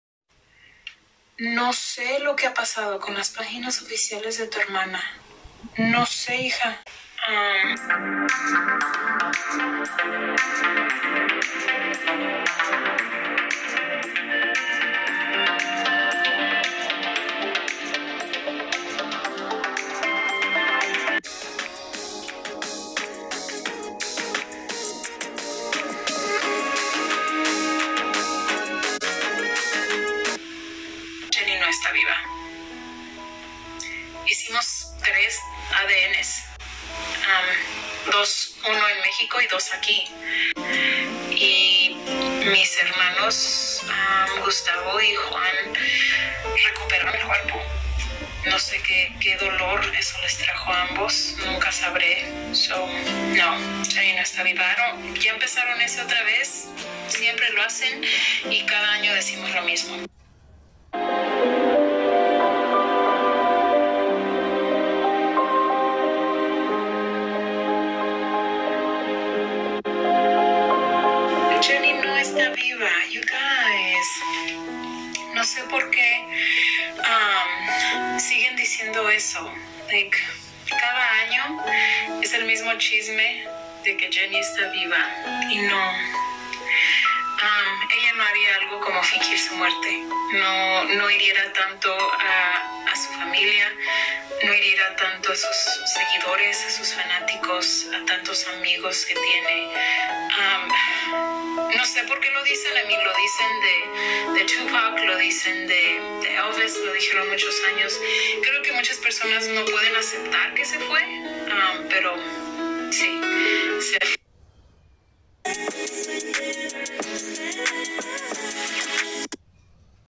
Rosie Rivera habla como nunca de la muerte de Jenni Rivera ¡! No vieron su cuerpo
Colocando un post, con la leyenda:
" JENNI RIVERA 2022 "

Es así como todos sus fanáticos, comenzaron a especular que Jenni, está viva, y en este año #2022, ella reaparecerá ¡!

Por tal motivo, su hermana Rosie Rivera habla como nunca de como lograron identificarla, sin tener el cuerpo al 100 por ciento.